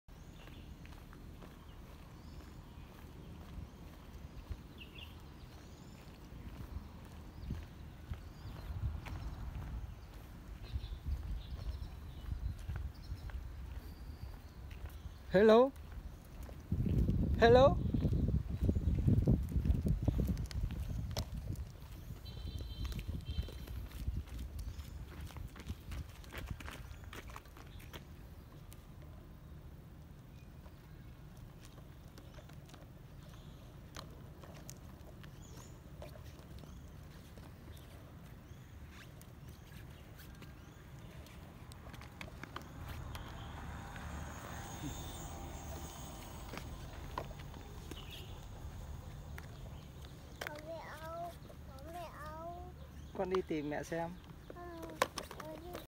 Đạp xe trong công viên
Búp đạp xe trong công viên, e mặc váy trắng như công chúa, đẩy xe như rùa